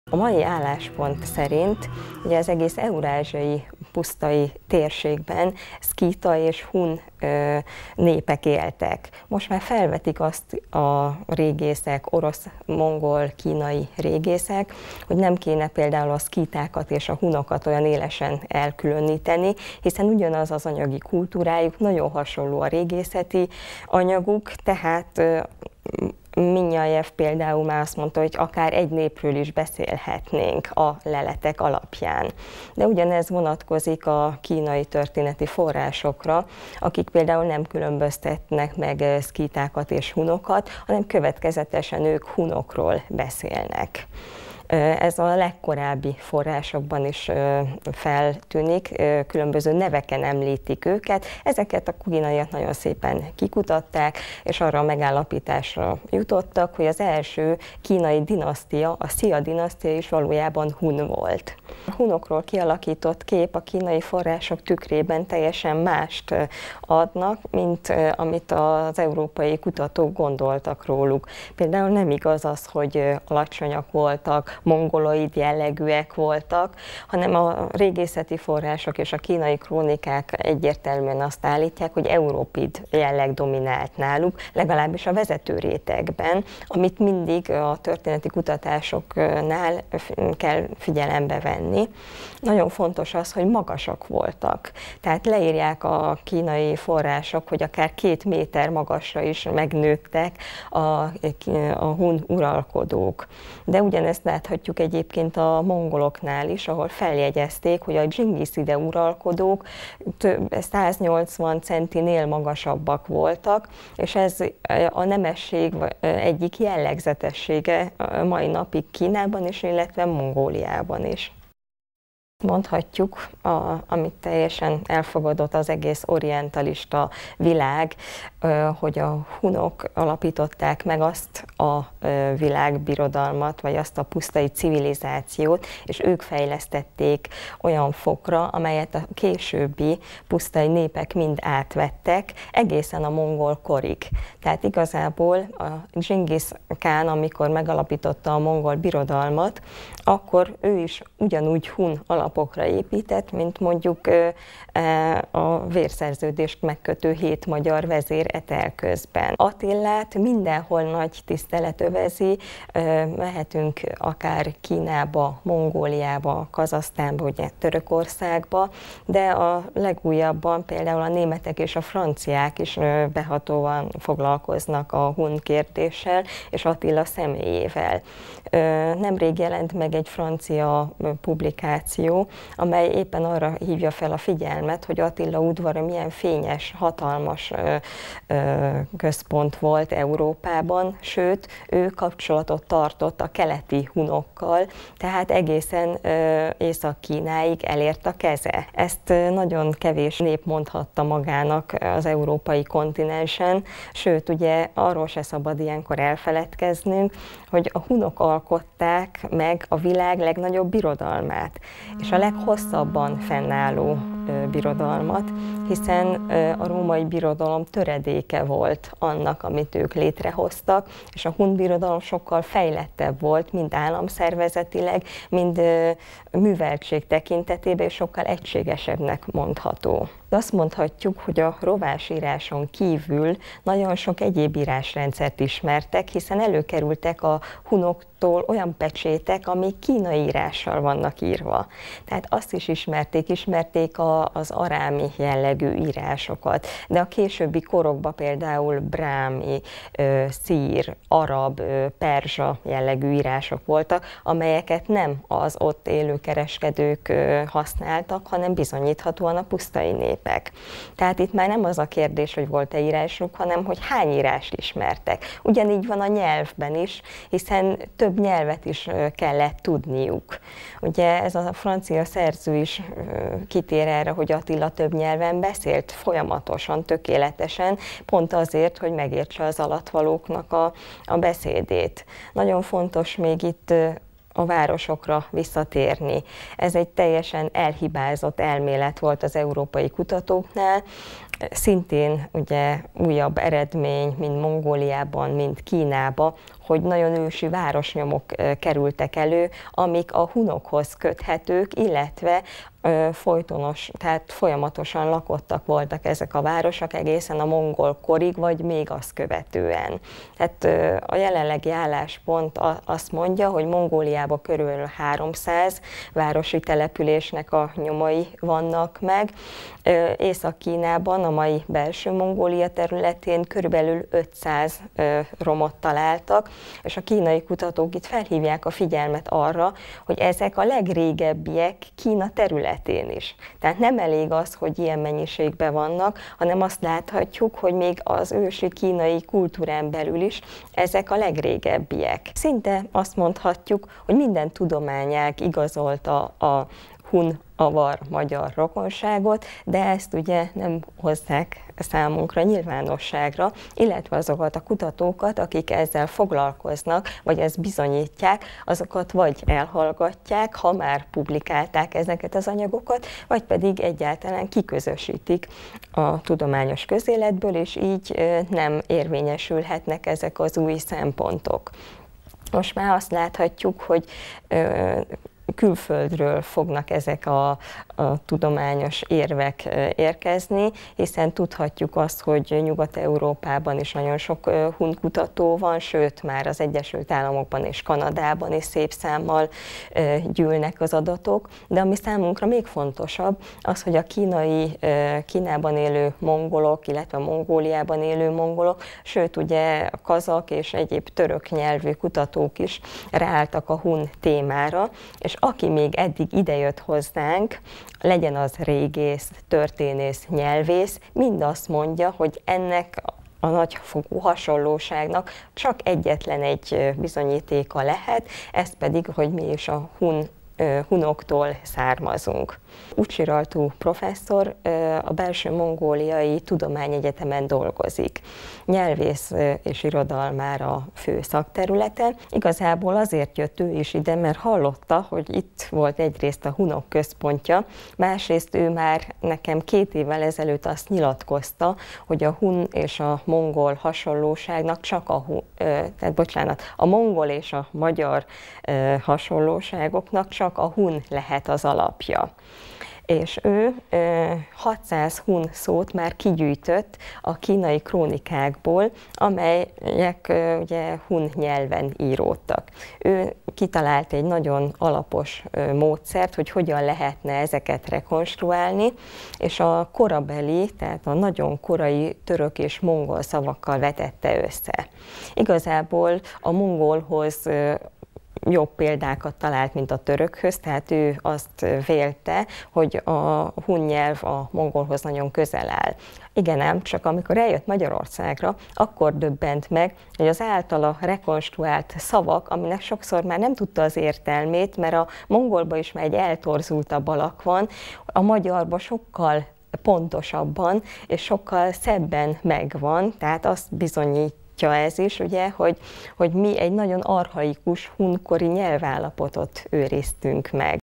Magyar - Hun Kapcsolat 2
A Magyar - Hun kapcsolatról beszél Obrusánszky Borbála történész, keleti kutató.  A hunok, a hun birodalom bemutatasa es kapcsolatuk a magyarokhoz.